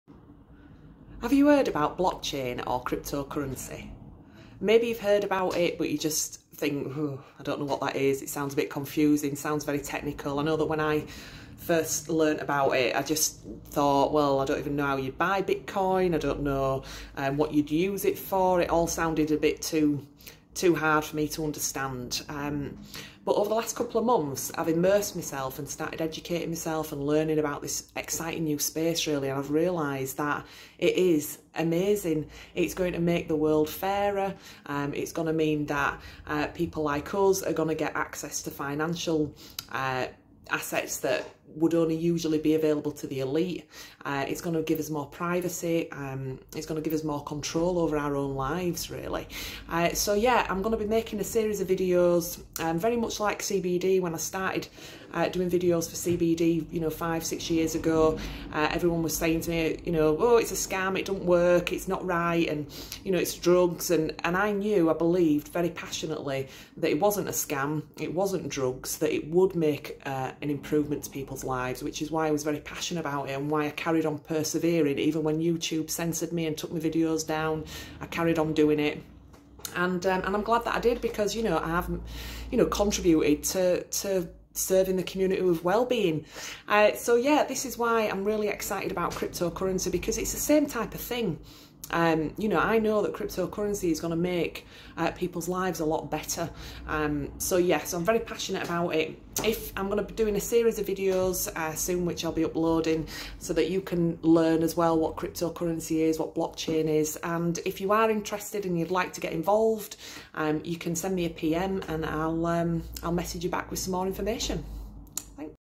Cryptocurrency Woman Welcome
Have you heard of blockchain and cryptocurrency ?
Maybe like me you had heard of it but didn't really understand it.🤔
I've spent the last couple of months educating myself about this new technology and I'm blown away by the positive impact it is having on society.😀
I'll be making a series of videos to share what I've learnt so you can benefit too.
If you want to know more drop me a PM 💚

Rachael Wilson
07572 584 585